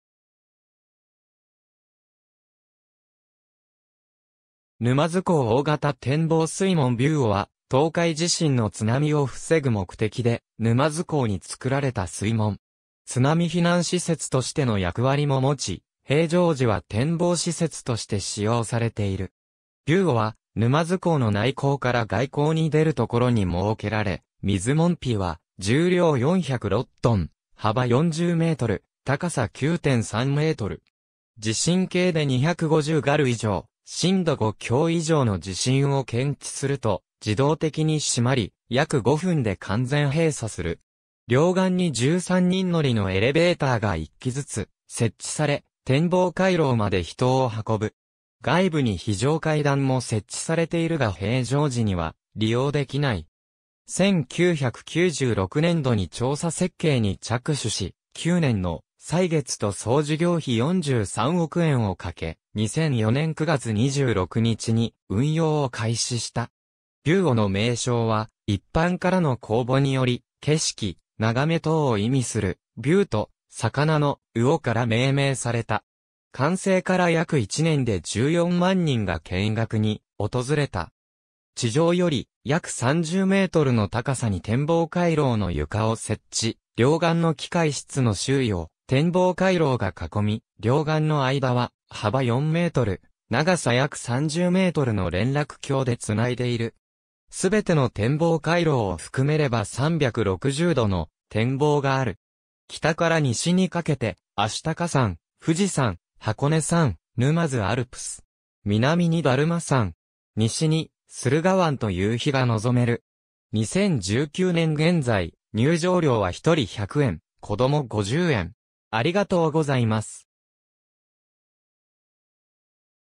びゅうお
沼津港大型展望水門びゅうお（ぬまづこうおおがたてんぼうすいもん-）は、東海地震の津波を防ぐ目的で沼津港に造られた水門。
津波避難施設としての役割も持ち、平常時は展望施設として使用されている。
びゅうおは、沼津港の内港から外港に出るところに設けられ、水門扉は重量406t、幅40m、高さ9.3m。
地震計で250ガル以上、震度5強以上の地震を検知すると自動的に閉まり、約5分で完全閉鎖する。
両岸に13人乗りのエレベーターが1基ずつ設置され、展望回廊まで人を運ぶ。
外部に非常階段も設置されているが平常時には利用できない。
1996年（平成8年）度に調査設計に着手し、9年の歳月と総事業費43億円をかけ、2004年（平成16年）9月26日に運用を開始した。
「びゅうお」の名称は、一般からの公募により景色・眺め等を意味する「VIEW」と魚の「うお」から命名された。
完成から約1年で14万人が見学に訪れた。
地上より約30mの高さに展望回廊の床を設置、両岸の機械室の周囲を展望回廊が囲み、両岸の間は幅4m、長さ約30mの連絡橋で繋いでいる。
全ての展望回廊を含めれば360度の展望がある。
北から西にかけて、愛鷹山、富士山、箱根山、沼津アルプス。
南に達磨山。
西に駿河湾と夕日が望める。
2019年現在、入場料は1人100円、子供50円。